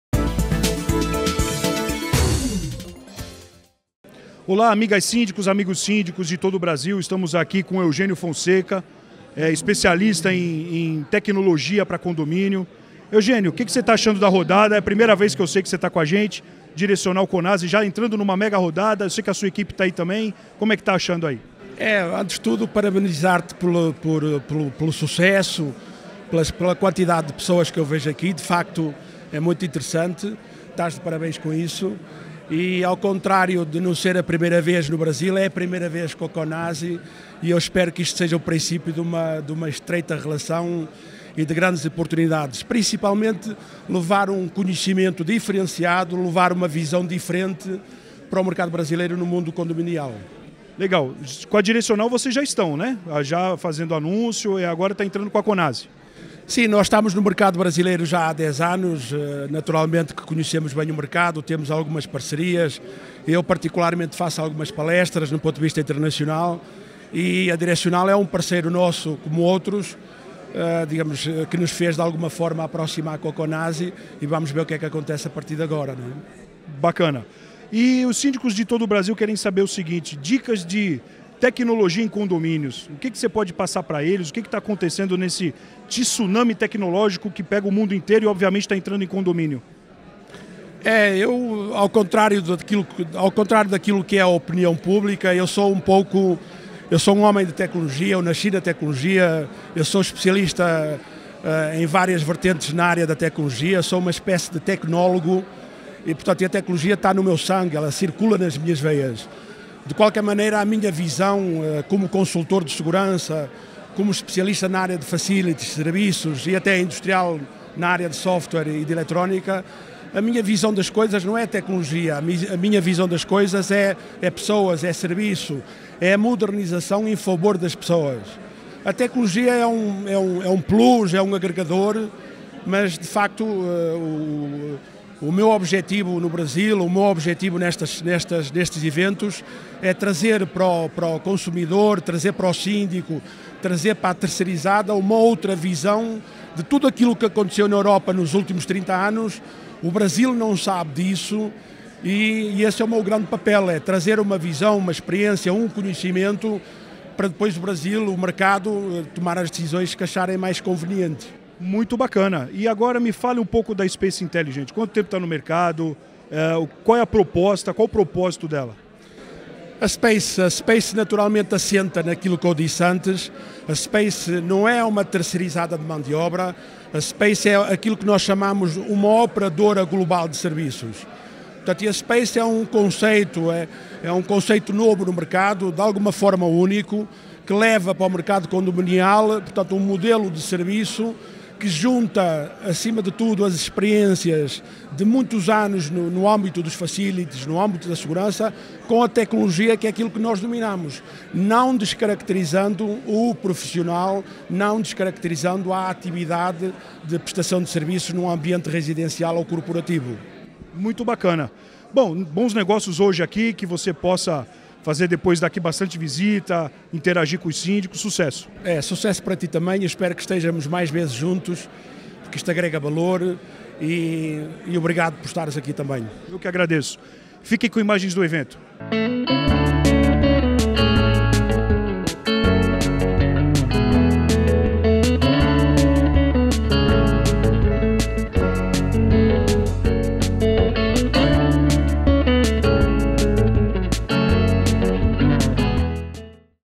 Dicas de Eugenio Fonseca - Especialista em Tecnologia - durante a Rodada de Negócios
Eugenio Fonseca - Especialista em Portaria Remota - durante a Rodada de Negócios - Direcional x CONASI, comenta sobre a futuro da Tecnologia nos condomínios em todo o Brasil, passando dicas essenciais aos Síndicos.
Eugênio também fala sobre o evento e sobre a proposta empresarial ao segmento condominial